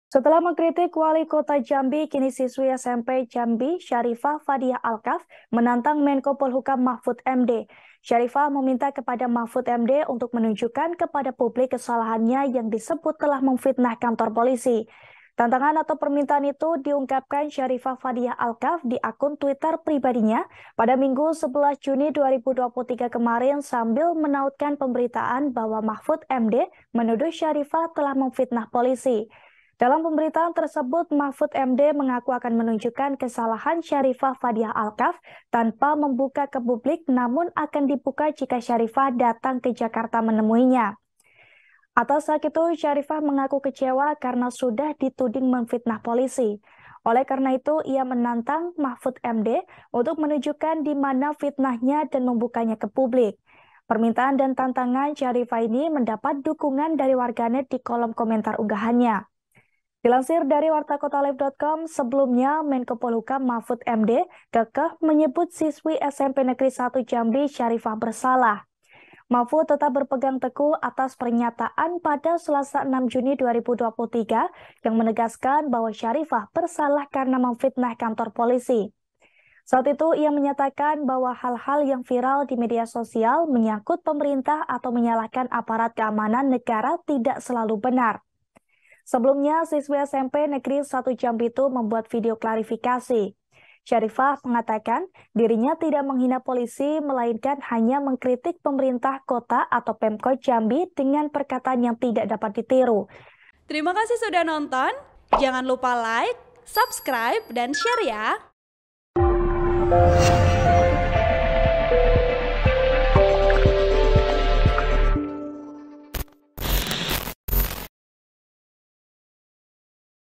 SFA Sampaikan Permintaan Khususnya pada Mahfud MD seusai Disebut Memfitnah Polisi
TRIBUN-VIDEO.COM - Siswi SMP Jambi Syarifah Fadiyah Alkaff yang mengkritik Wali Kota Jambi Syarif Fasha soal kerjasama Pemkot Jambi dan perusahaan China yang proyeknya merugikan masyarakat kini menantang Menko Polhukam Mahfud MD menunjukkan ke publik kesalahannya yang disebut telah memfitnah kantor polisi.

Tantangan atau permintaan itu diungkapkan siswi SMP Jambi Syarifah Fadiyah Alkaff di akun Twitternya @dydyaa2, Minggu (11/6/2023), sambil menautkan pemberitaan bahwa Mahfud MD menuduh Syarifah telah memfitnah polisi.

Selain itu dalam pemberitaan tersebut Mahfud MD mengaku akan menunjukkan kesalahan siswi SMP Jambi Syarifah Fadiyah Alkaff tanpa membuka ke publik namun akan dibuka jika Syarifah datang ke Jakarta menemuinya.

Atas hak itu, Syarifah mengaku kecewa karena sudah dituding memfirnah polisi. Karenanya ia menantang Mahfud MD menunjukkan dimana fitnahannya dan membukanya ke publik.

Baca berita Wartakotalive.com lainnya di Google News

Artikel ini telah tayang di WartaKotalive.com dengan judul Siswi SMP Jambi Syarifah Tantang Mahfud MD Buka ke Publik Soal Tudingan Fitnah Polisi, Tak Tertutup,
Penulis: Budi Sam Law Malau | Editor: Budi Sam Law Malau